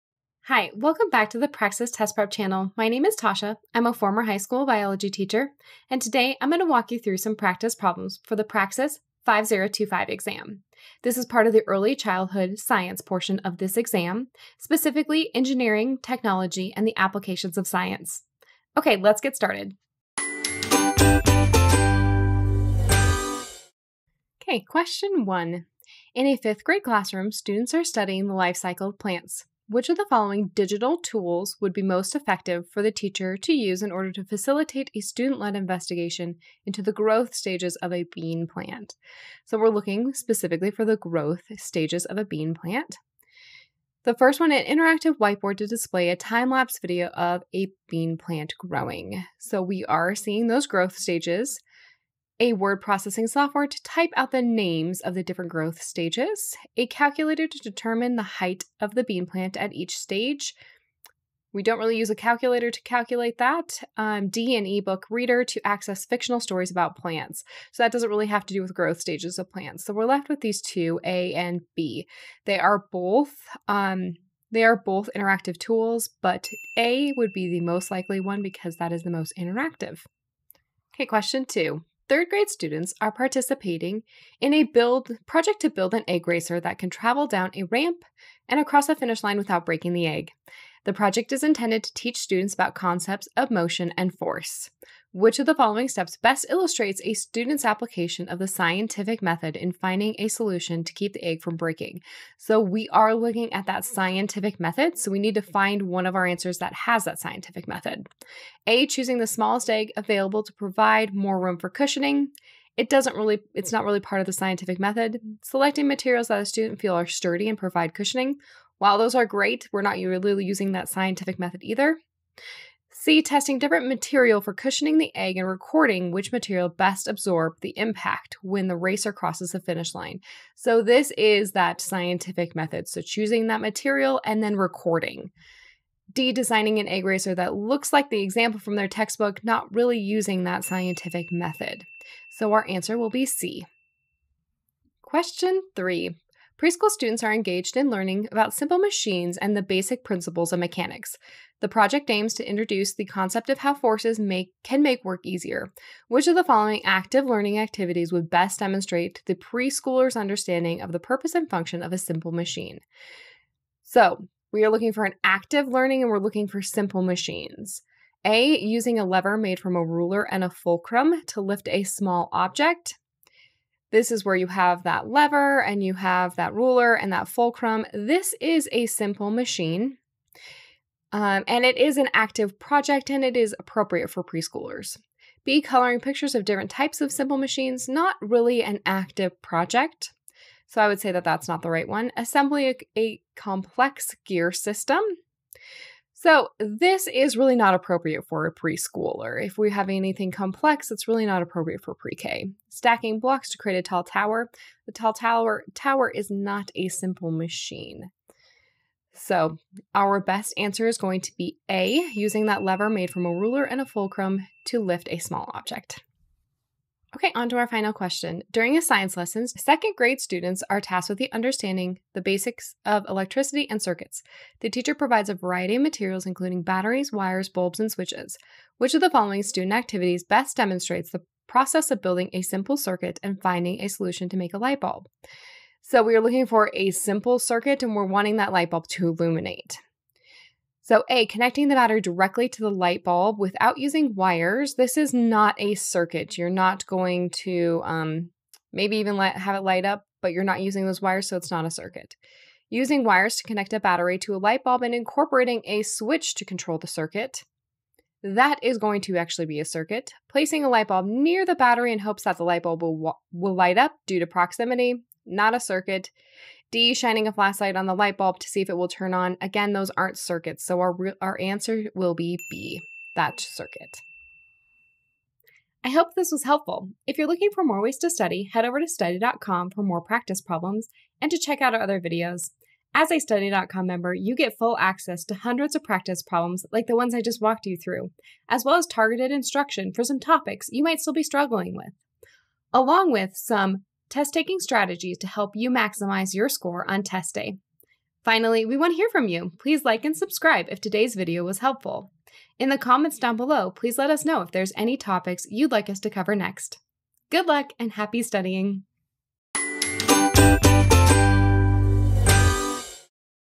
Applications of Science Practice Problems for Praxis Early Childhood Education (5025)
Looking for authentic Engineering, Technology, and Applications of Science practice problems for the Praxis Early Childhood Education Exam (5025)?

Join former high school Biology teacher and Praxis Coach - Tasia - as we walk you through the types of Engineering, Technology and Applications of Science questions that you are likely to face. This video will provide step-by-step explanations for each question as well as strategy tips to help you save time & tackle any Engineering, Technology and Applications of Science question that comes your way.

🔗 LINKS 🔗

🔎 CHAPTERS 🔎

00:00 - Intro
00:25 - Problem #1
01:21 - Problem #2
02:26 - Problem #3
03:36 - Problem #4